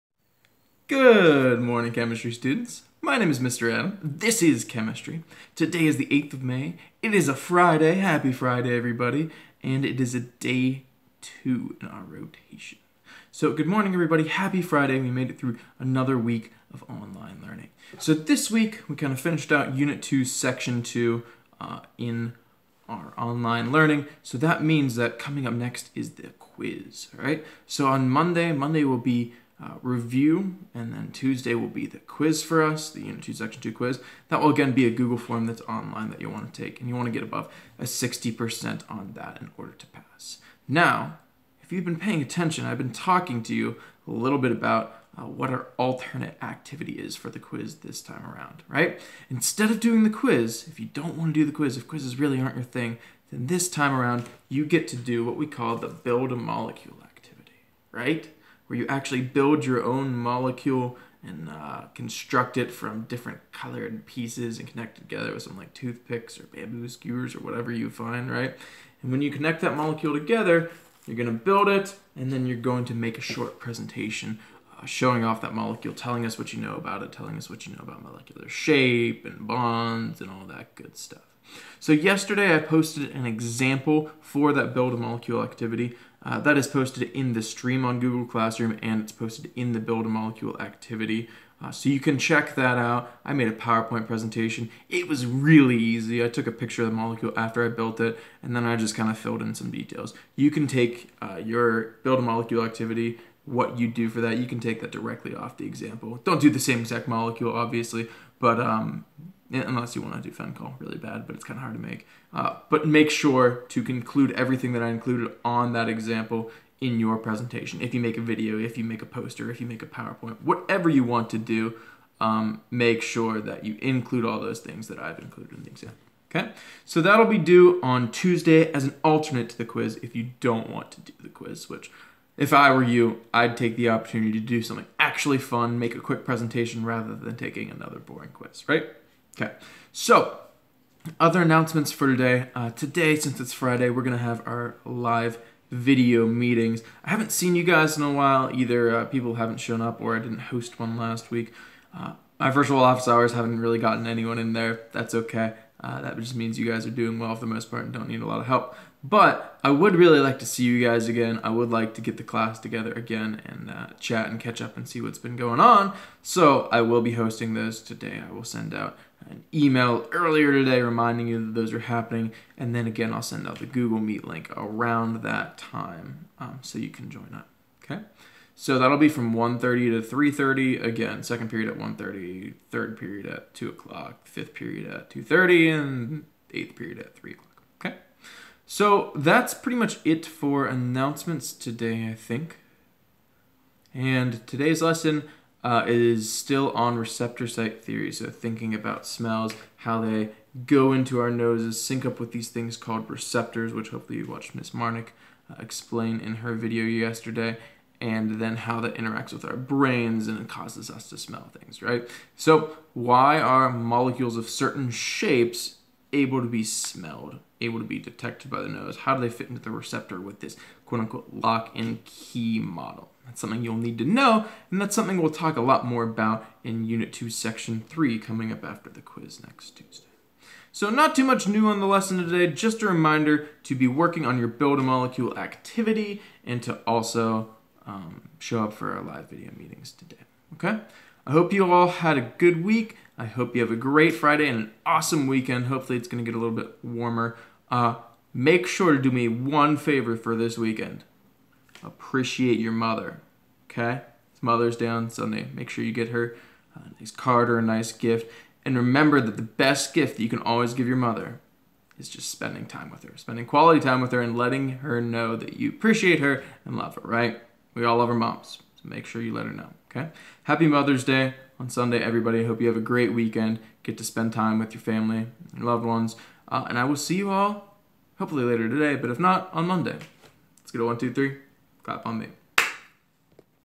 Day 30 - Receptor Site Theory Part 2
Good morning, chemistry students!  My name is Mr. Adam, this is chemistry, and welcome to Day 30 of online learning!

Daily Video #30 - Daily video meetings today, hoping to see some of you that I haven't seen for a while!  Today, we end Unit 2 Section 2, which means our quiz will be next Tuesday.  If you don't want to do the quiz, do the "Build-A-Molecule" activity instead!  That will also be due next Tuesday - check out the example I posted yesterday.  Happy Friday - make sure to appreciate your mother this weekend!

VIDEO MEETINGS TODAY:
2nd Period: 1:30 - 2:00
3rd Period: 2:00 - 2:30
5th Period: 2:30 - 3:00
8th Period: 3:00 - 3:30